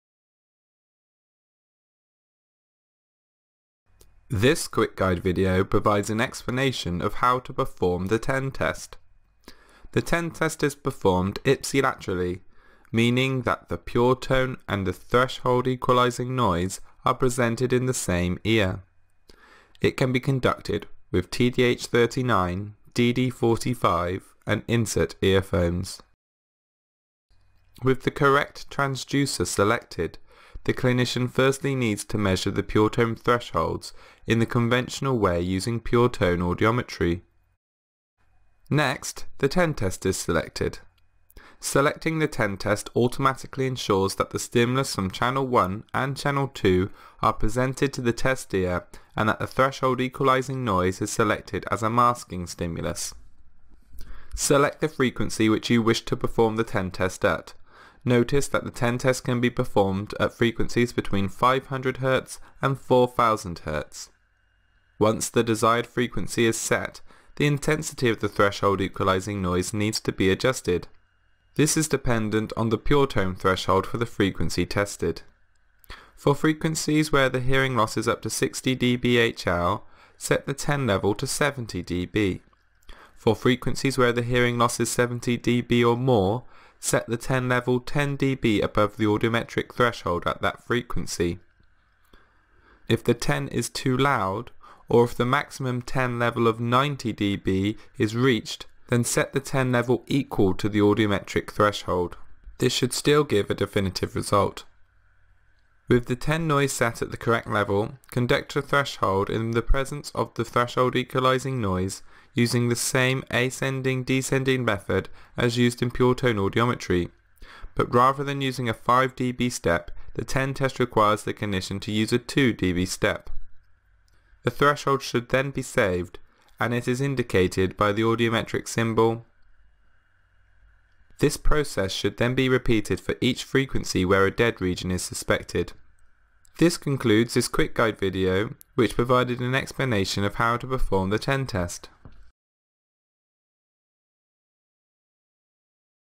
TEN Test: Procedure
This video explains how to perform the threshold equalizing noise (TEN) test.

Timestamps:
0:00 Introduction
0:09 Stimuli and transducers
0:27 Pure tone audiometry
0:38 TEN test procedure

To learn more about the TEN test, head to our guide